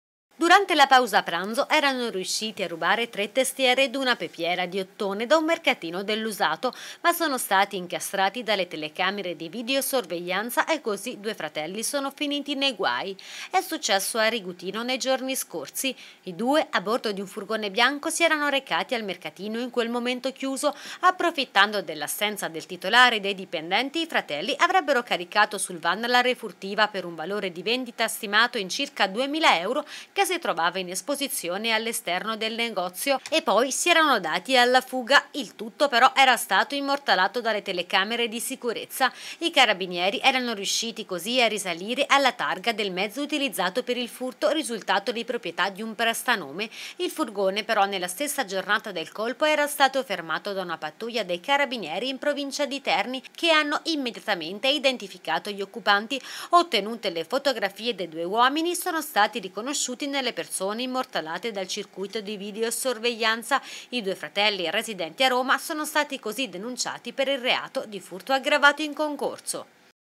Furto al mercatino dell'usato: denunciati due fratelli
I carabinieri di Arezzo hanno denunciato in stato di libertà per furto aggravato in concorso due fratelli residenti a Roma dopo un furto di tre testiere ed una pepiera da letto in ottone per un valore di vendita stimato in circa 2.000 euro.